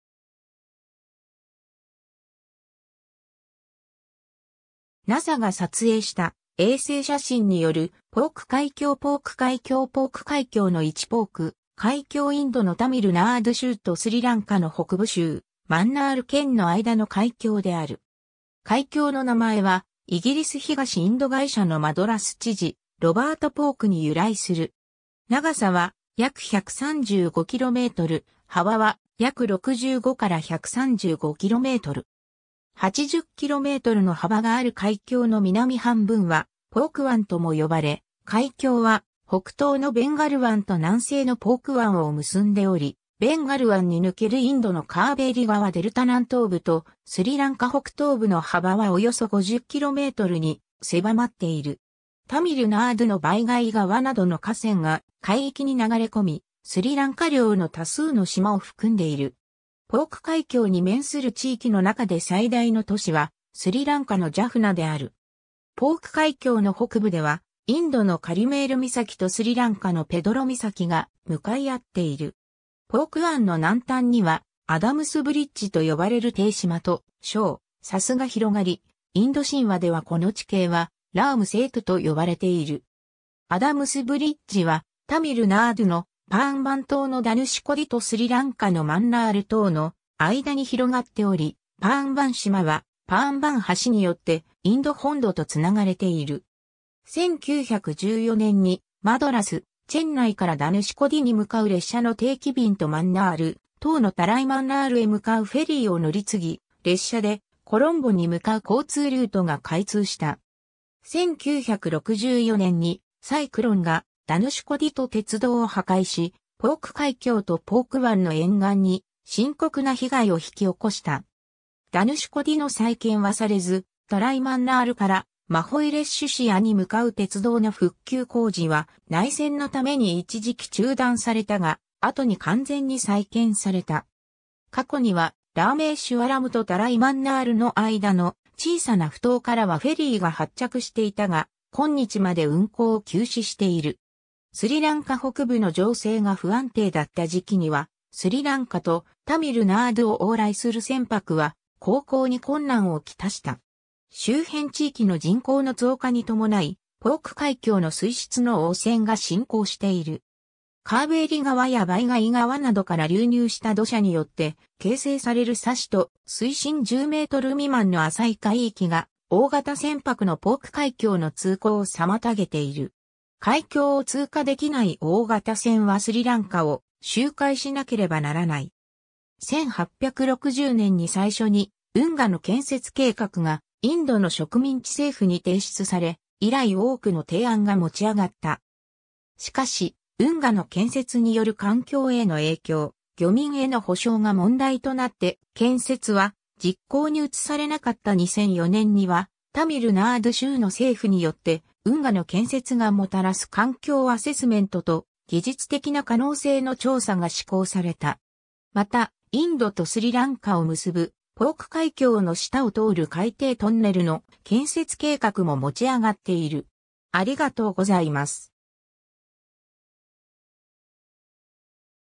ポーク海峡
NASAが撮影した衛星写真によるポーク海峡 ポーク海峡 ポーク海峡の位置 ポーク海峡（シンハラ語: පෝක් සමුද්‍ර සන්ධිය 、タミル語: பாக்கு சலசந்தி / பாக்கு நீரிணை, Pok Samudra Sandhiya）インドのタミル・ナードゥ州とスリランカの北部州マンナール県の間の海峡である。
海峡の名前はイギリス東インド会社のマドラス知事ロバート・ポーク（任期：1755年 - 1763年）に由来する。
 長さは約135km、幅は約65-135km。
80kmの幅がある海峡の南半分はポーク湾とも呼ばれ、海峡は北東のベンガル湾と南西のポーク湾を結んでおり、ベンガル湾に抜けるインドのカーヴェーリ川デルタ南東部とスリランカ北東部の幅はおよそ50kmに狭まっている。
タミル・ナードゥのヴァイガイ川などの河川が海域に流れ込み、スリランカ領の多数の島を含んでいる。
 ポーク海峡に面する地域の中で最大の都市は、スリランカのジャフナである。
 ポーク海峡の北部ではインドのカリメール岬とスリランカのペドロ岬が向かい合っている。
 ポーク湾の南端には、アダムスブリッジと呼ばれる低島と礁、砂州が広がり、インド神話ではこの地形はラーム・セートゥ（「ラーマの橋」の意）と呼ばれている。
アダムスブリッジはタミル・ナードゥのパーンバン島（ラーメーシュワラム島）のダヌシュコディとスリランカのマンナール島の間に広がっており、パーンバン島はパーンバン橋によってインド本土と繋がれている。
 1914年にマドラス/チェンナイからダヌシュコディに向かう列車の定期便とマンナール島のタライマンナールへ向かうフェリーを乗り継ぎ、列車でコロンボに向かう交通ルートが開通した（en）。
1964年にサイクロンがダヌシュコディと鉄道を破壊し、ポーク海峡とポーク湾の沿岸に深刻な被害を引き起こした。
ダヌシュコディの再建はされず、タライマンナールからMahawilachchiyaに向かう鉄道の復旧工事は内戦のために一時期中断されたが、後に完全に再建された。
過去にはラーメーシュワラムとタライマンナールの間の小さな埠頭からはフェリーが発着していたが、今日まで運航を休止している。
 スリランカ北部の情勢が不安定だった時期には、スリランカとタミル・ナードゥを往来する船舶は航行に困難をきたした。
 周辺地域の人口の増加に伴い、ポーク海峡の水質の汚染が進行している。
 カーヴェーリ川やヴァイガイ川などから流入した土砂によって形成される砂嘴と水深10m未満の浅い海域が、大型船舶のポーク海峡の通行を妨げている。
海峡を通過できない大型船はスリランカを周回しなければならない。
1860年に最初に運河の建設計画がインドの植民地政府に提出され、以来多くの提案が持ち上がった。
しかし、運河の建設による環境への影響、漁民への保障が問題となって建設は実行に移されなかった2004年にはタミル・ナードゥ州の政府によって運河の建設がもたらす環境アセスメントと、技術的な可能性の調査が施行された。
 また、インドとスリランカを結ぶ、ポーク海峡の下を通る海底トンネルの建設計画も持ち上がっている。